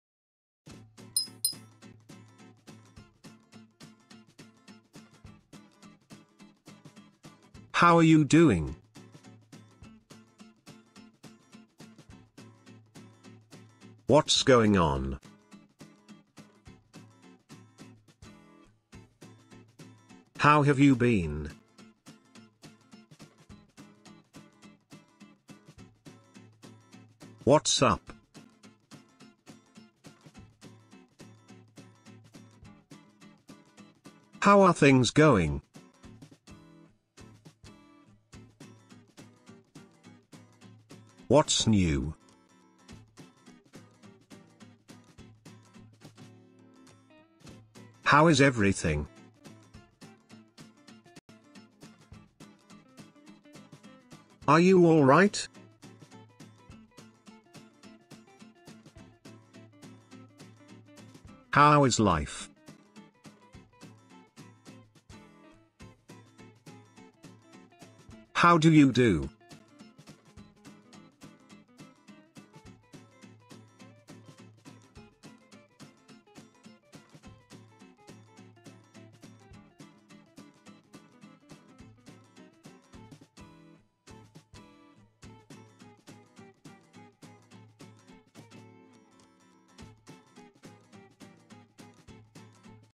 TEN WAYS TO SAY 'HOW ARE YOU?' IN ENGLISH
In this video, we are going to learn alternatives to "How are you?". This way, you can expand your vocabulary and avoid repeating the same words again and again.

What’s up?

How are things going?

What’s new?

How is eveything?

How is life?